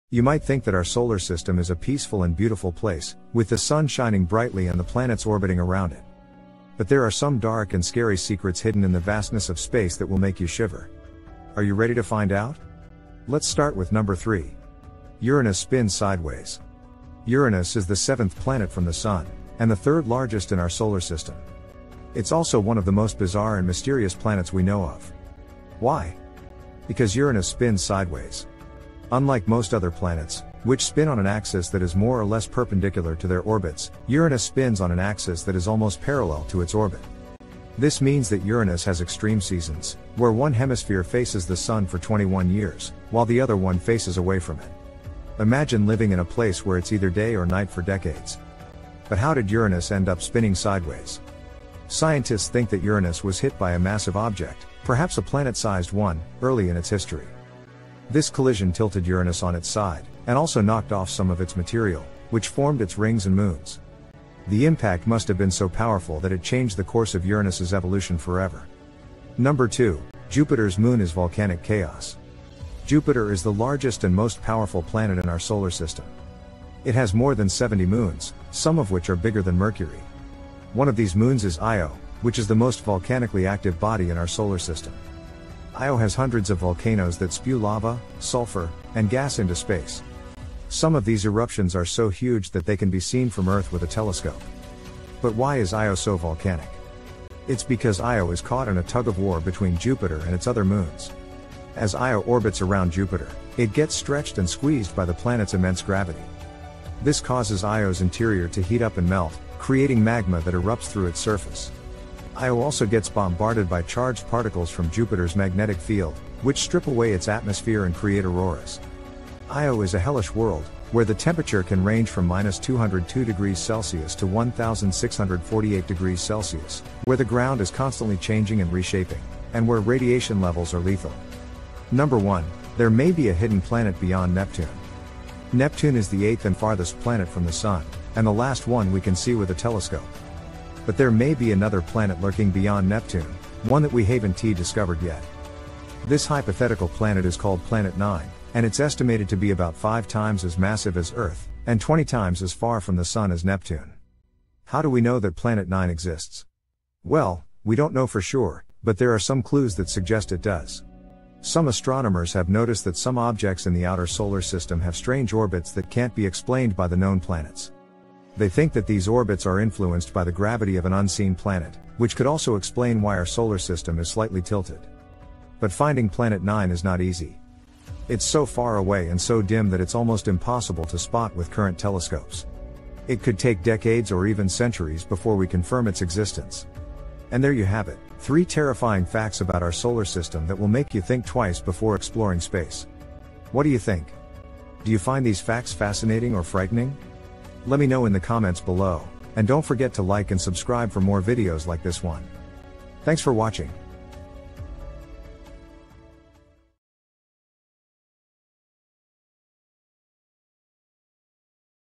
3 Terrifying Facts About Our Solar System
In this video, I’m going to share with you some of the most terrifying facts about our solar system that will make you rethink your place in the universe. You’ll learn about a planet that spins sideways, a moon that erupts with lava, and a hidden planet that could be lurking beyond Neptune. These facts are so scary that they might give you nightmares. Watch this video at your own risk!